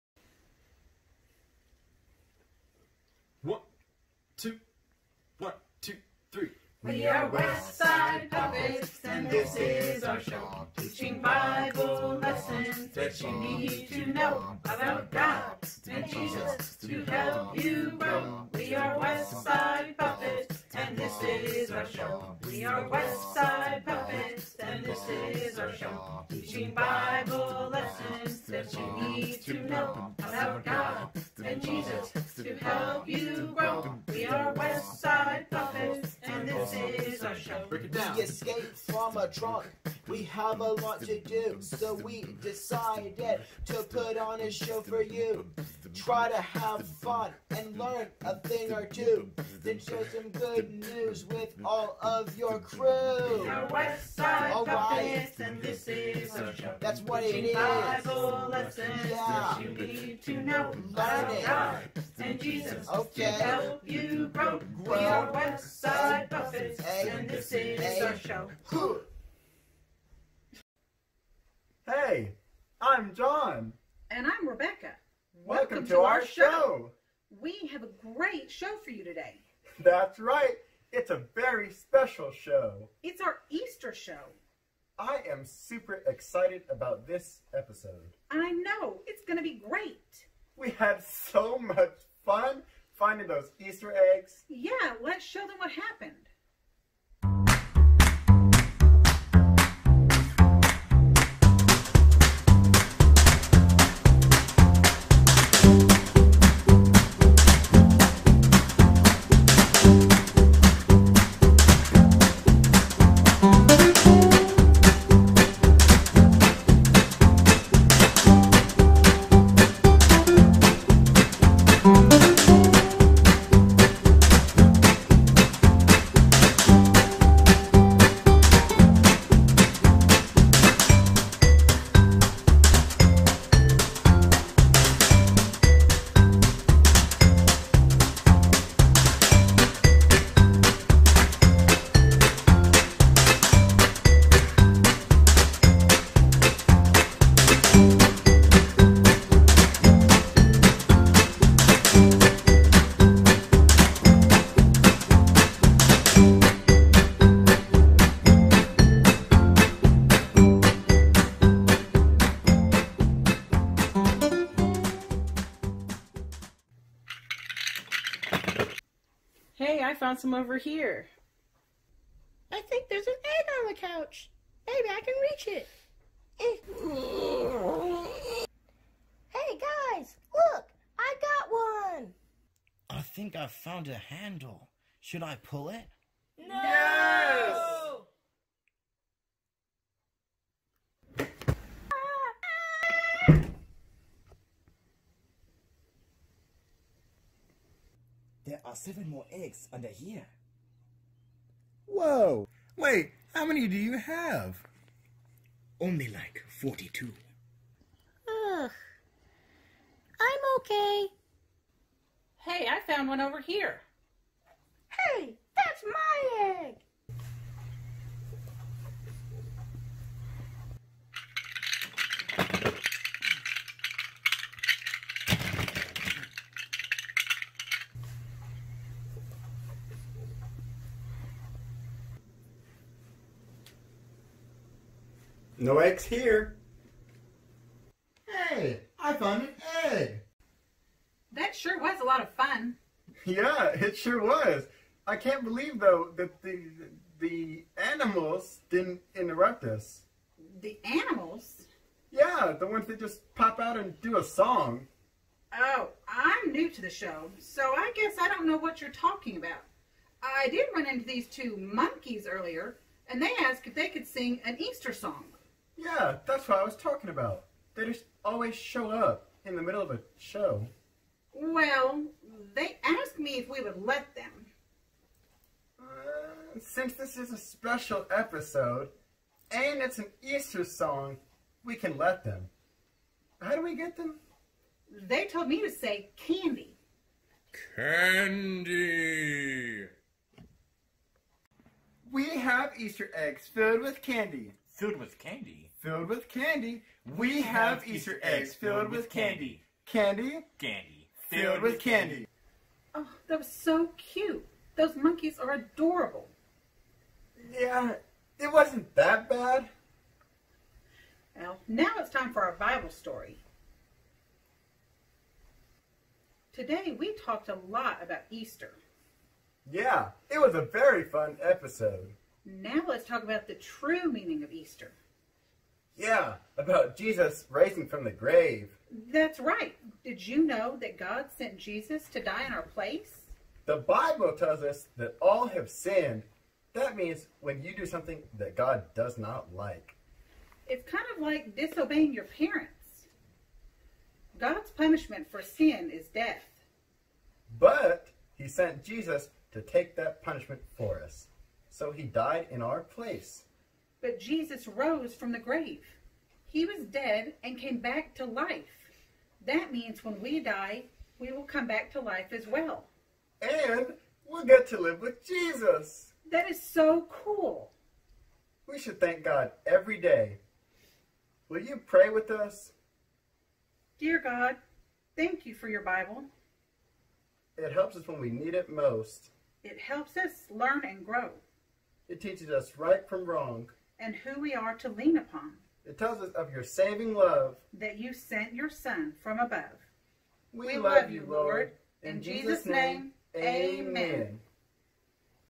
Westside Puppets Episode 3 Easter Show
It is our Easter Episode.  Today our puppets have a great time hunting Easter Eggs and discussing the true meaning of Easter.  Jesus died for us and rose again.  We hope you enjoy the show.